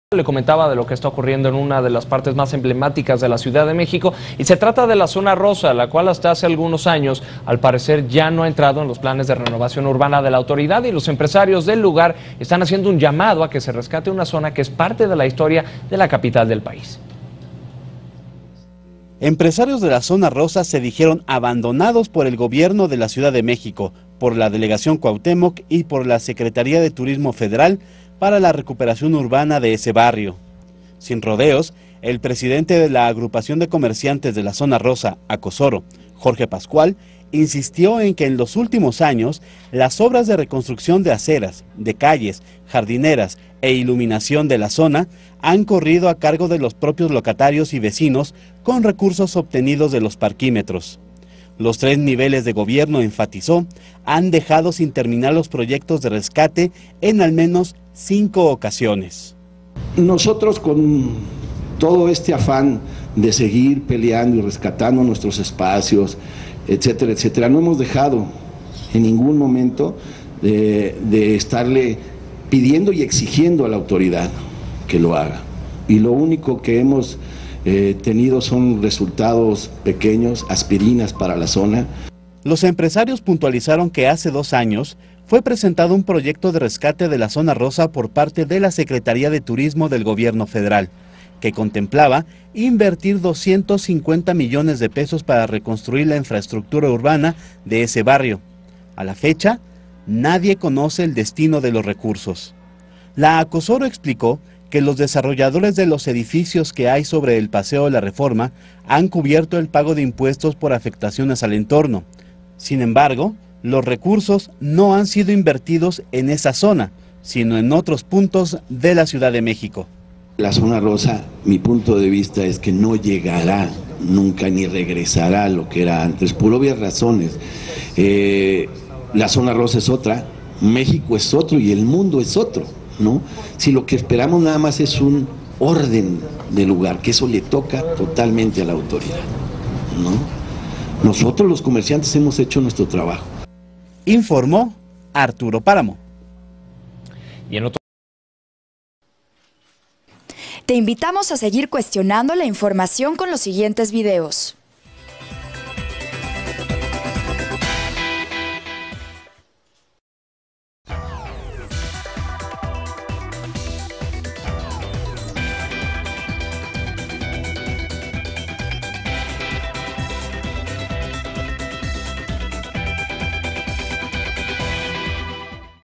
Rescates urbanos ignoran Zona Rosa / Ricardo Salas
Habitantes de la Zona Rosa denuncian que el programa de Rescates Urbanos de la Ciudad de México, ha olvidado la rehabilitación de esta importante parte de la capital del país.

05 de julio 2016

Te invitamos a ver el siguiente video:

Carlos Puente y las modificaciones a la Ley 3 de 3